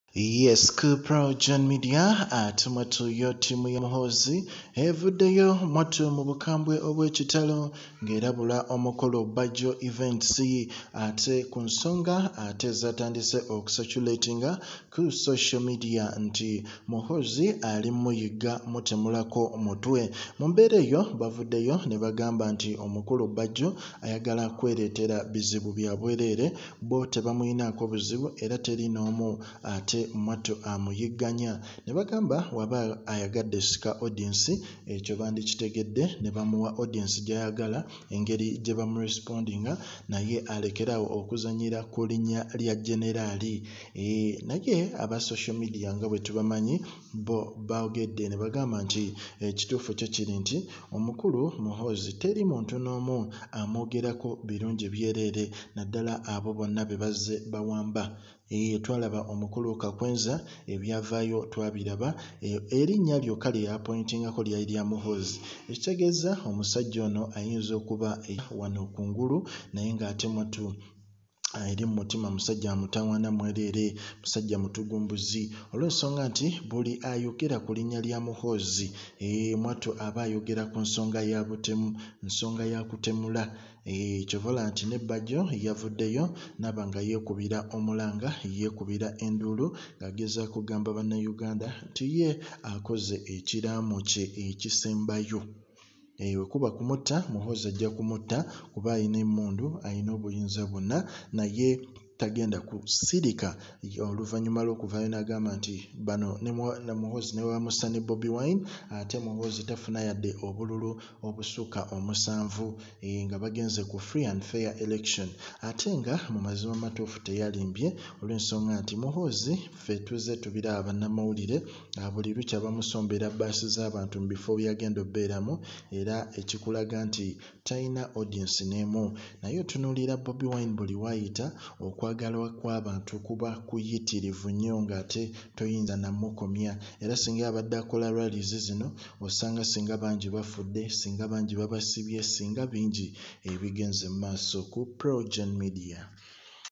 Giwunye Ebyokumutemula muhoozi Alabuddebowa Bobi wine Bajjo Okusasanya Obutambi Ekiddako
Giwunye Ebyokumutemula muhoozi Alabudde Bajjo Okusasanya Obutambi Ekiddako ojjakyiwulira
Kitalo Muhoozi Alayidde Okumutemula Owa Bobi wine lwamuyisaamu maaso Bajjo naye tatidde mwetegefu
Entiisa Ne muhoozi Ayagala kumutemula Wuno Owa Bobi wine yekubide enduulu kumbera yadukaduka jalimu